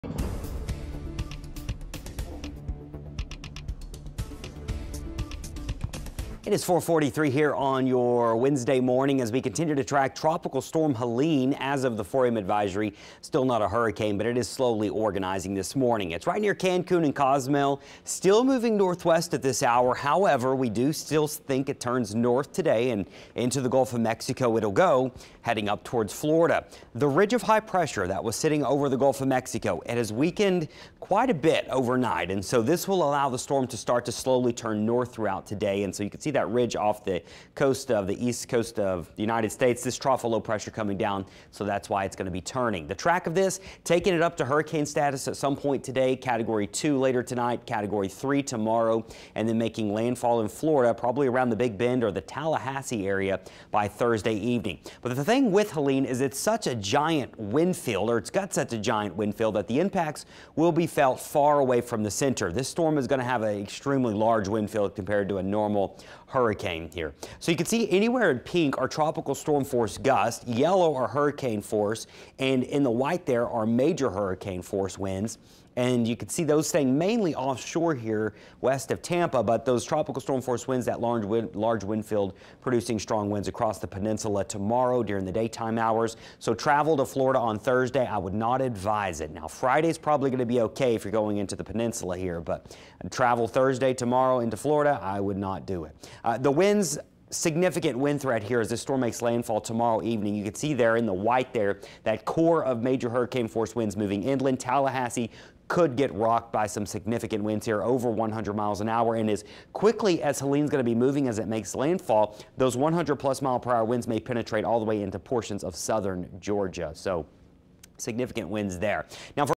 Wednesday 5 AM Tropical Update: Helene expected to become a hurricane Wednesday
Helene is nearing Cancun and the Yucatan Channel on Wednesday morning. The storm is slowly intensifying as it grows an inner core.

The motion is northwest but should turn northerly later Wednesday as the ridge over the Gulf of Mexico weakens. This will allow Helene to enter the Gulf later today and begin to speed up Thursday. The storms will be under low wind shear, and very hot water temps.

Intensification could become rapid Thursday as the storm nears Florida.

Landfall will likely be Thursday evening in the Big Bend as a major hurricane. Helene will have a giant wind field, so impacts will stretch across a large portion of Florida throughout Thursday. The storms will quickly move inland into Georgia Thursday night bringing more rain and gusty winds.